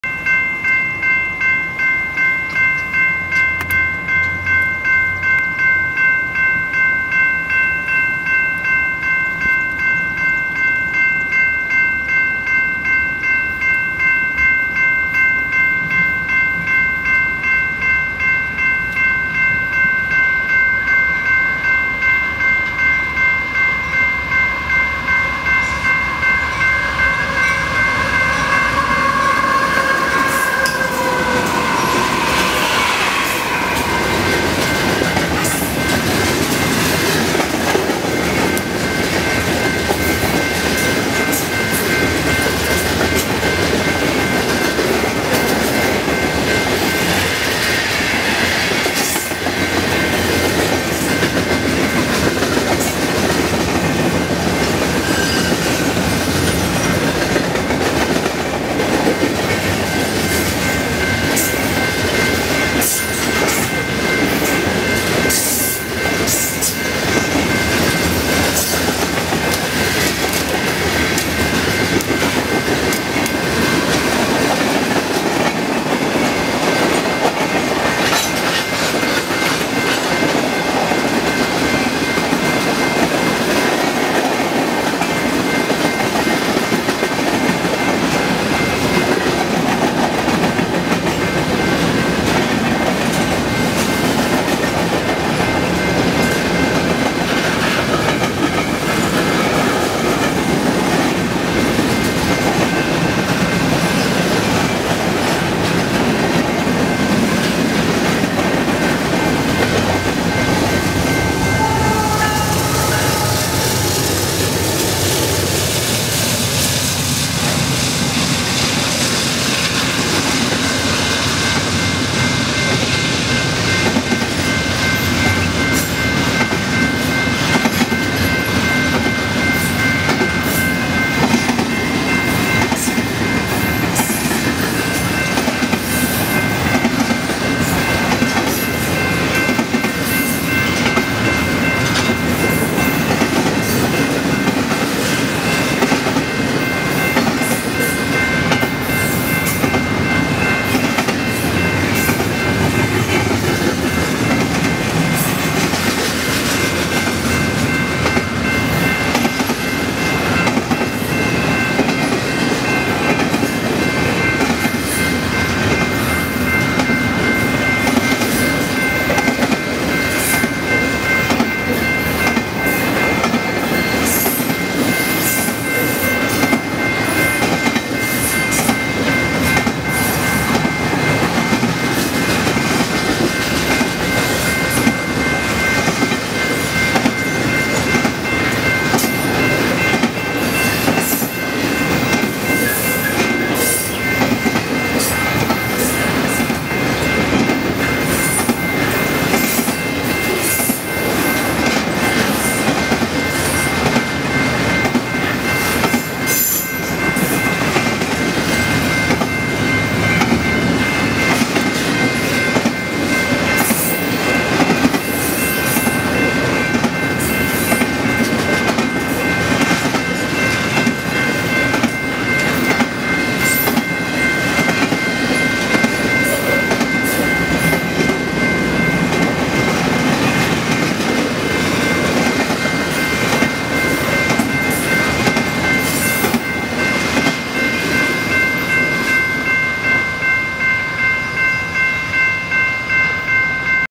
EB Manifest & WB Intermodal Olathe, KS 5-10-14 11:09am
Videos taken along the Emporia Subdivision in Olathe, KS
"AKA" The Transcon

Video was taken on May 10, 2014 at 11:09am at the Santa Fe Street Crossing West of Kansas Avenue on Emporia Sub in Olathe.

EB: BNSF 5617, BNSF 5249 & BNSF 7010
WB: BNSF 6877, BNSF 4146 & BNSF 4073